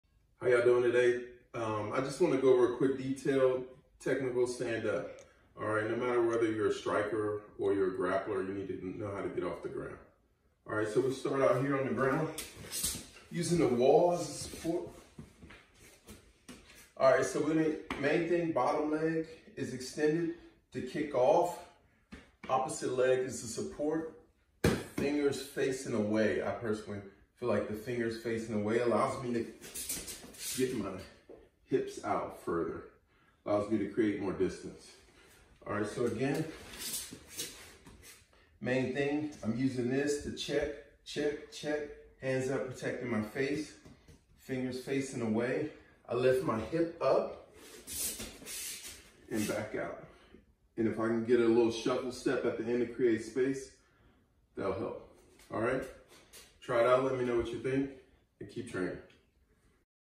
Technical standup
1) Bottom leg extended
2) Raise Hips☝🏾
3) Keep Guard Up! martialartsminute mma jiujutsu 🥋